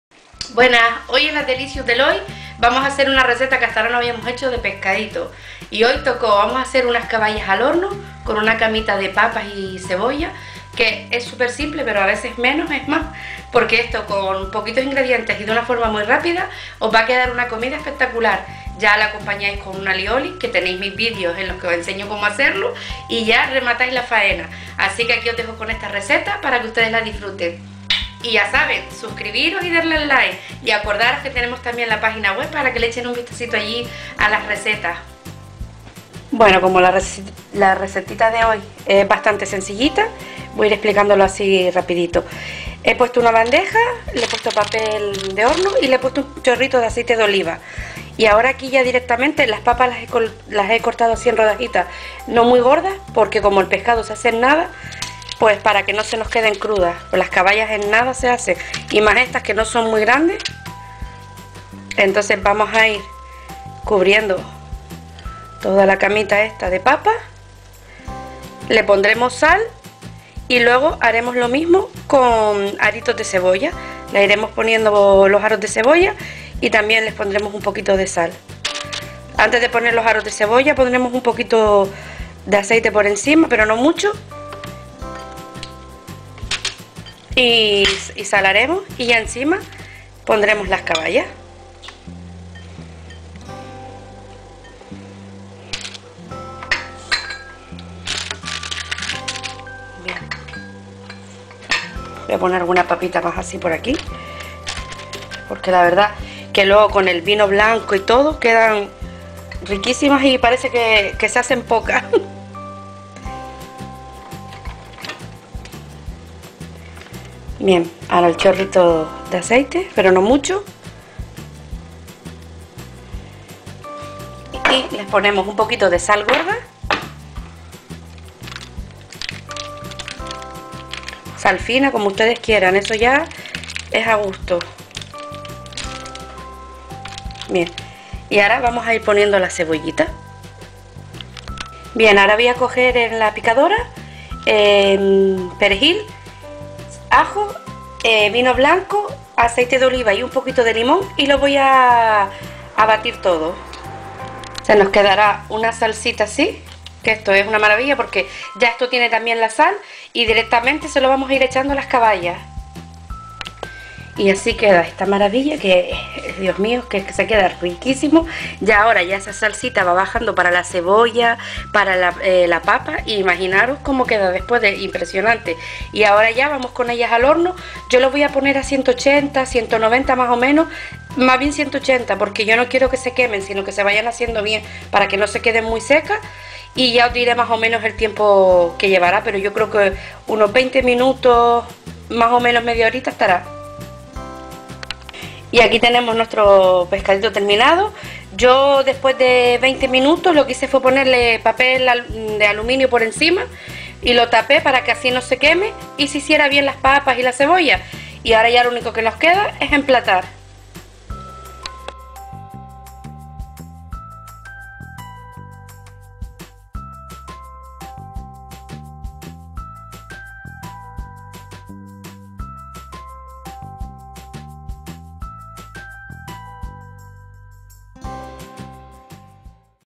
las mejores caballas al horno
Te gusta el pescado pero que la cocina no huela tanto como al freírlo? De esta manera es fácil y rápida y no te dejará la casa con el olor del pescado!